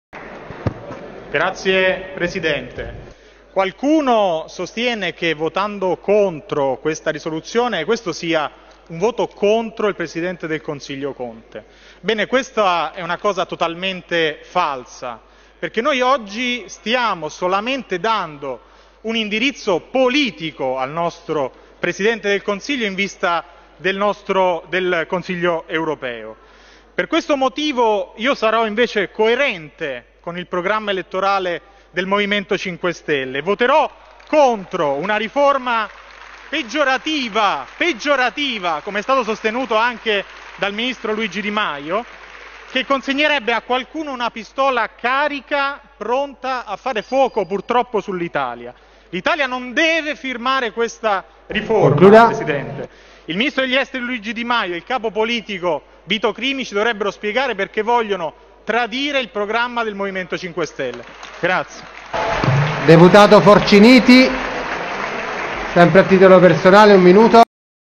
Berardini, uno dei pochissimi deputati del M5S ad aver votato ‘no’ al Mes (10.12.20)
- Roma - L’intervento di Berardini, uno dei pochissimi deputati del M5S ad aver votato ‘no’ alla riforma del Mes (10.12.20)